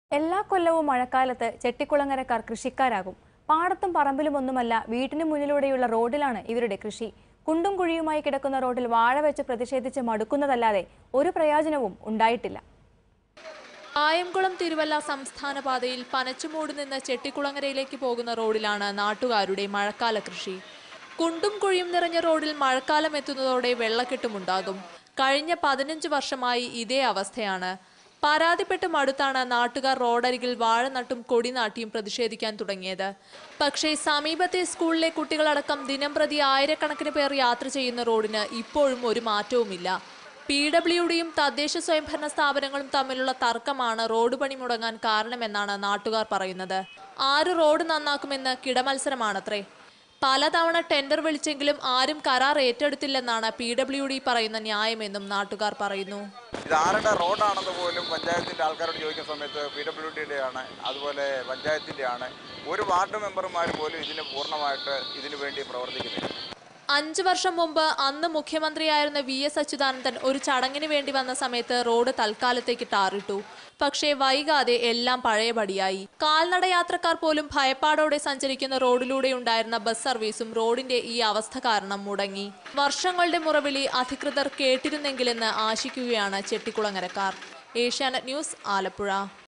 Alappuzha News:Locals begin farming in road: Chuttuvattom 8th June 2013 ചുറ്റുവട്ടം
Locals begin farming in road